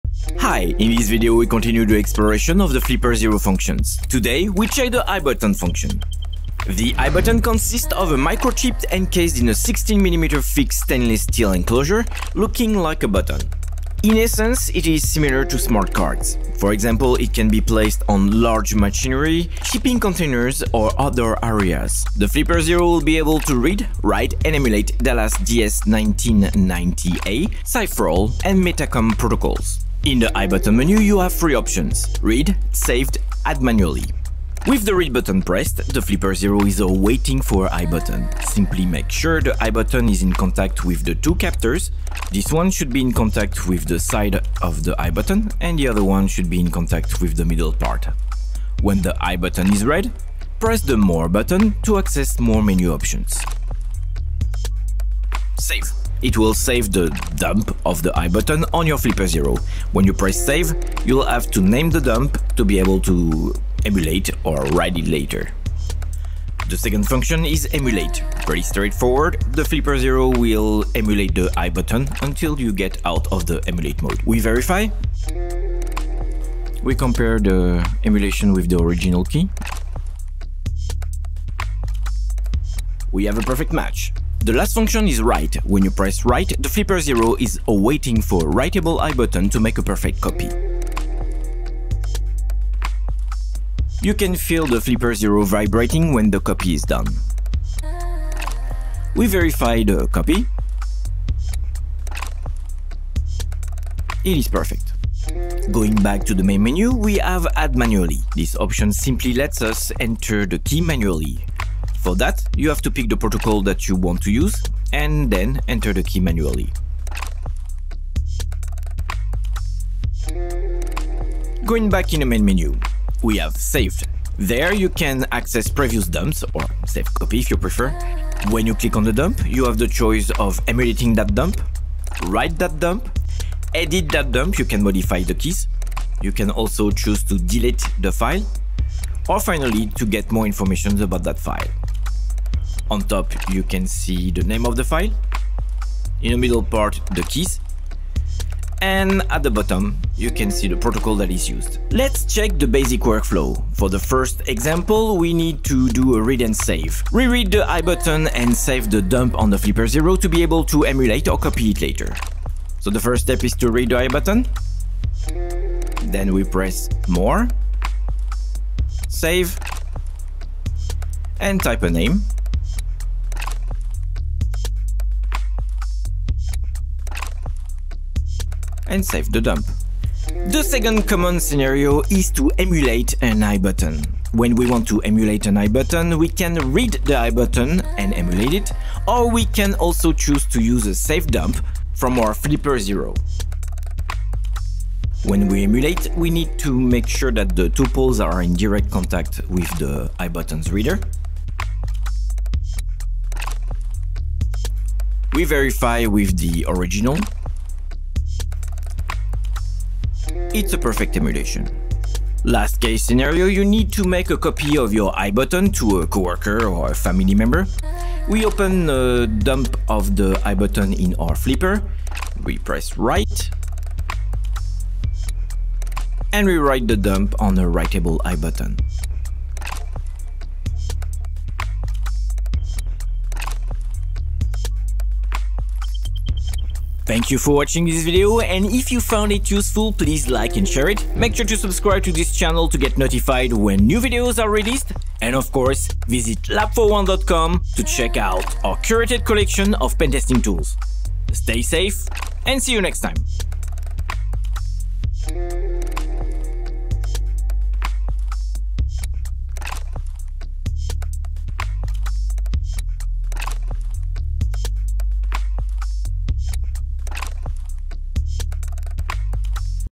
pentestips flipperzero, everything you need to know about the iButton function
In this video, we continue the exploration of the flipper zero functions. Today we check the ibutton function and show you all you need to know about it!
Learn how to read, emulate, and write an ibutton.